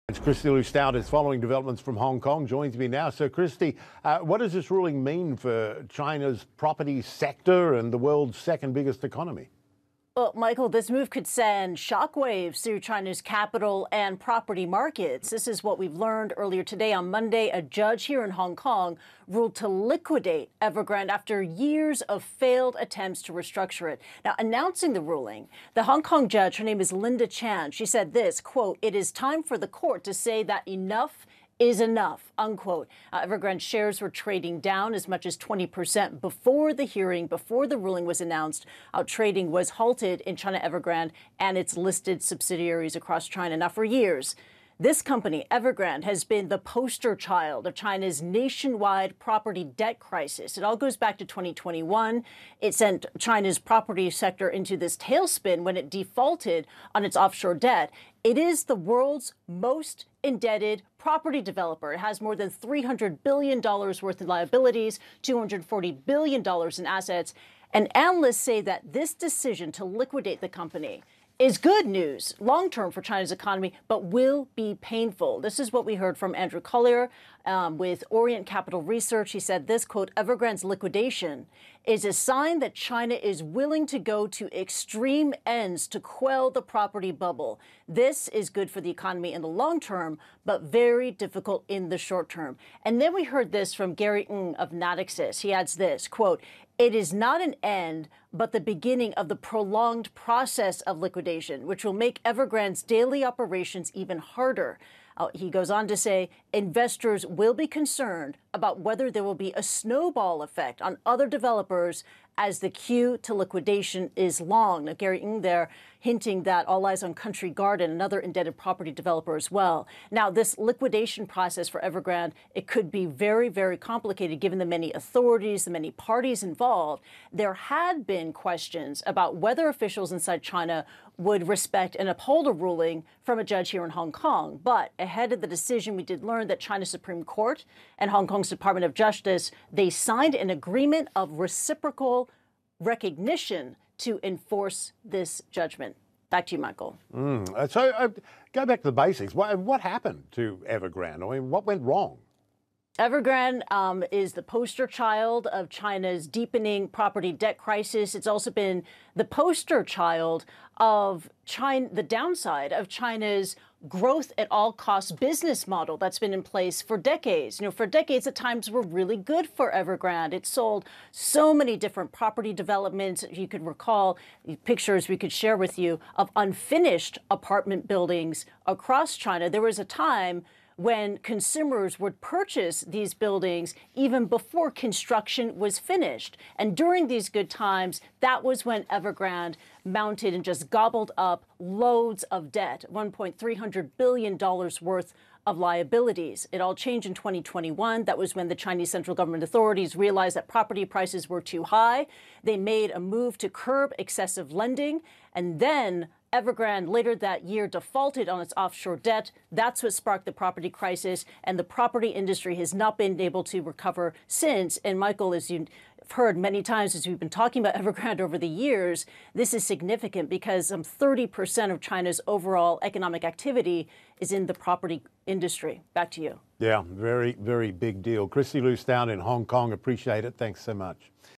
Evergrande, symbol of China’s property crisis, heads to liquidation
Evergrande, the world’s most indebted property developer, was ordered to be liquidated by a Hong Kong court on Monday after years of failed attempts to restructure it. Announcing the ruling, the judge said: "It is time for the court to say enough is enough." The Shenzhen-based developer has become a poster child of China’s property crisis since 2021, with total liabilities of over $300 billion. Analysts say the decision is good news for China's economy in the long run, but will be painful.  CNN's Kristie Lu Stout reports.